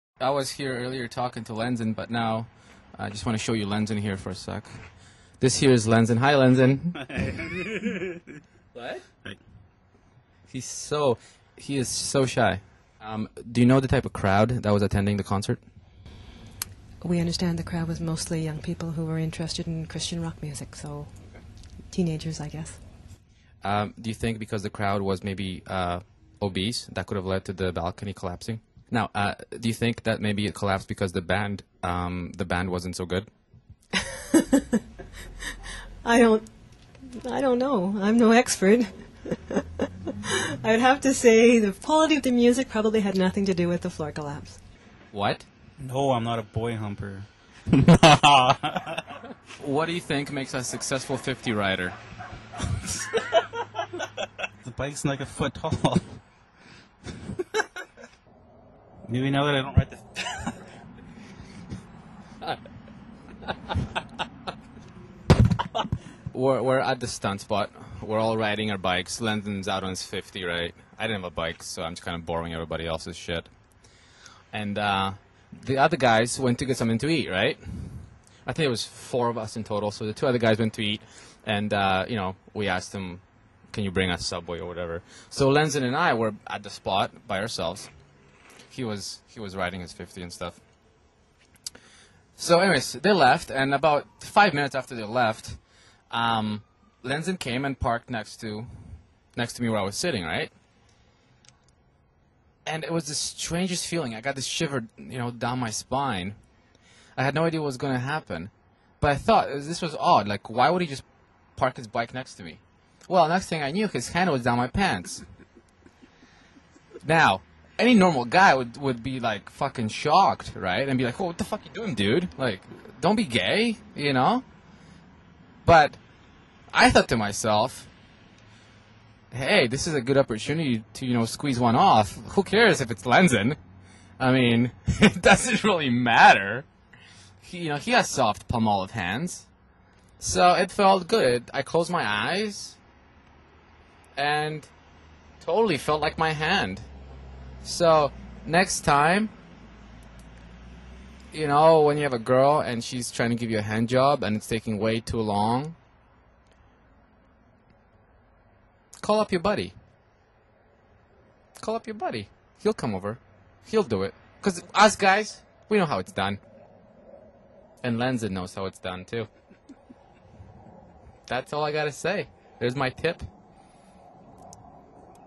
Hip Hop Sexy Masturbation
Just silly shit plus im talking to a lawyer about what happend with the church balcony fallin' down on the jebus people..